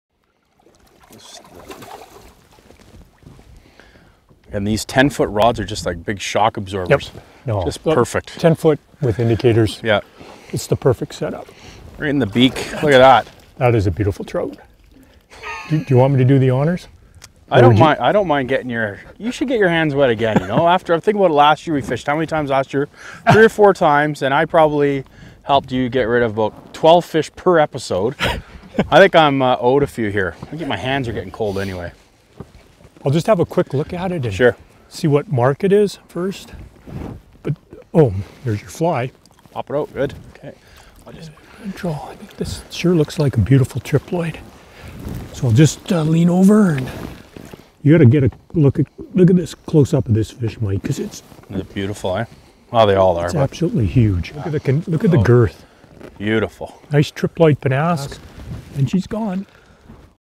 Fishing for Brown Trout
The pages of BCs oldest and most trusted outdoors publication BC Outdoors magazine, comes alive in high definition on a brand new fishing program.Join Host and editor Mike Mitchell as he and several different Co Hosts fish through out BC chasing Trout, Salmon, Steelhead and Giant White Sturgeon in this action packed and informative 30 minutes of fishing heaven. Along the way you will be treated to our learning with the pros segments and information on the gear you need to be successful out on the water.